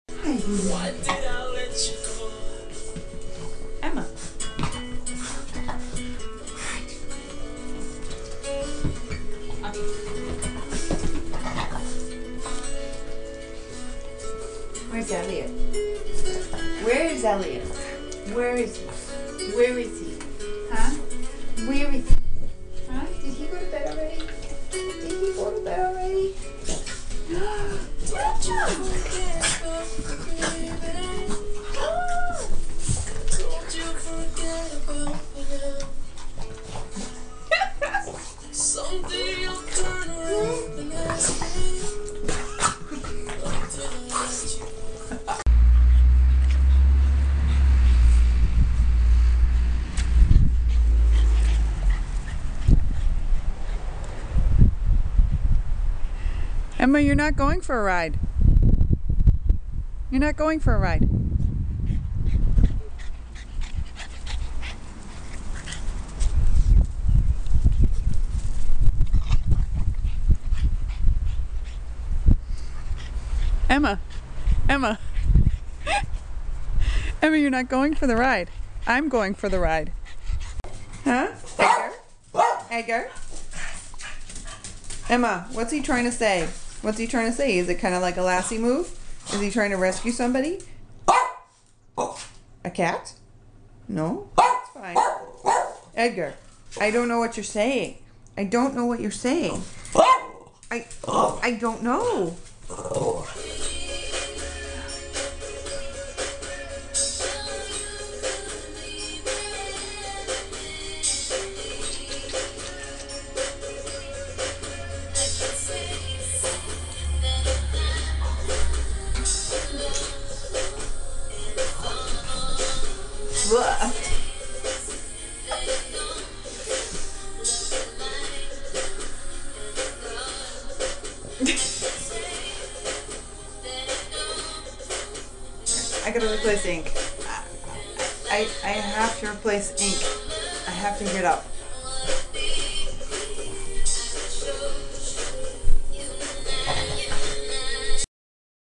Dogs050610.flv
My dogs are my constant and entertaining companions.  I am amazed at how different each of them is...behaviors, movement, sound, and attitudes.  Although none of them have ever had any formal training, they are all very good at responding to requests, going with me everywhere, and riding/living in the car for hours.  One of the reasons I got the little G11 is because it has a video capability.  I wanted to make short video clips of my dogs.  When I had Sammy, all I had when he died in 2005 were a few still photos.  I wanted more than that...I wanted to remember how he sounded and moved and acted, and I struggle to recall it as time goes by.  So, with these 3 dogs I will have that!